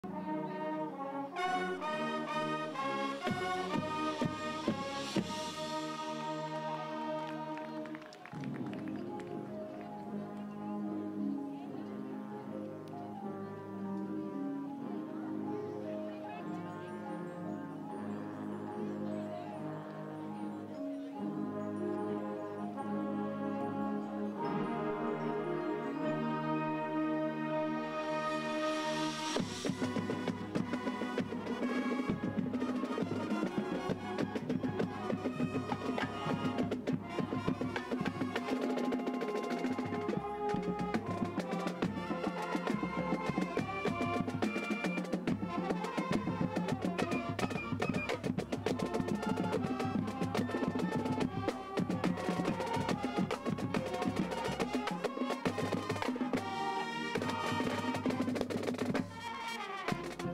Band #1 – Colonial High School
Colonial High School’s marching band is competing in week 10 of WFTV’s Battle of the Bands.